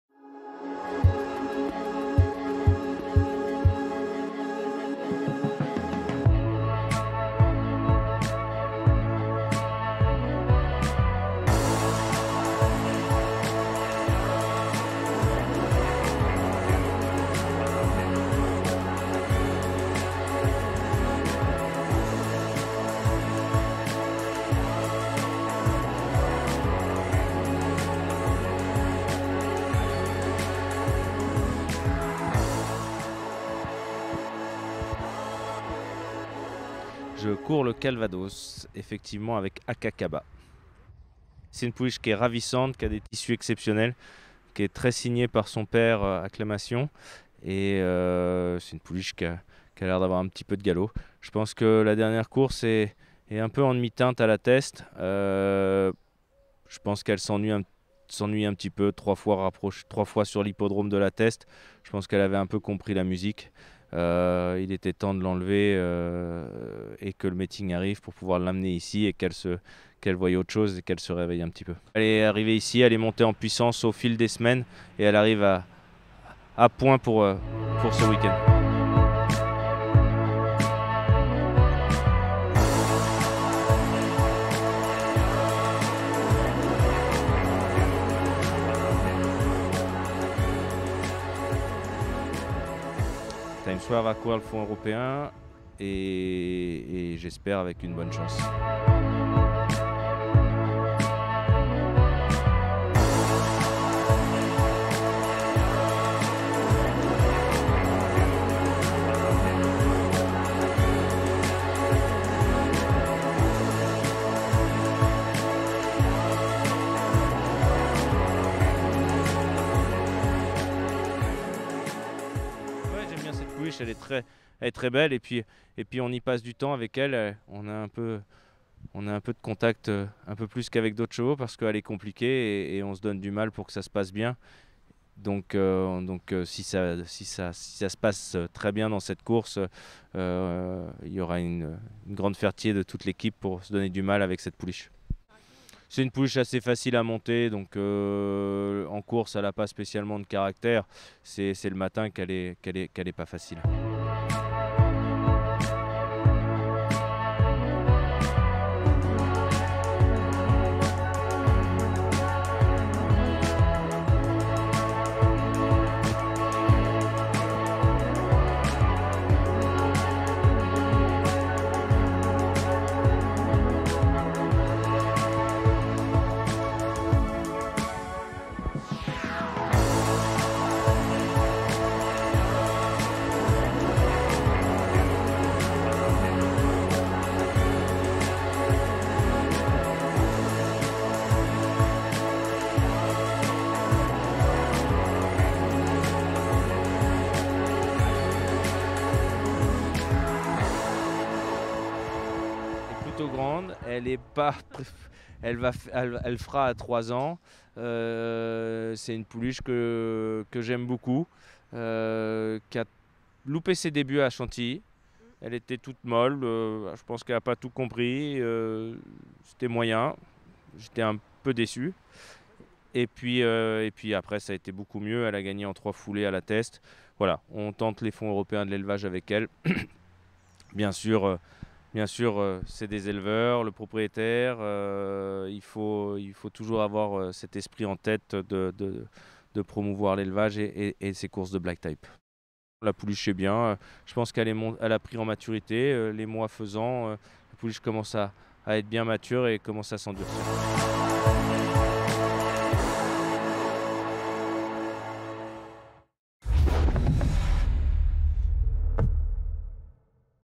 CHRISTOPHE FERLAND À L'ASSAUT DE LA CÔTE NORMANDE | Reportage
Christophe Ferland présente 3 chevaux samedi 21 août à Deauville :
- Accakaba dans le Prix du Calvados
- Times Square et Bahasa dans le Critérium du Fonds Européen de l'Élevage.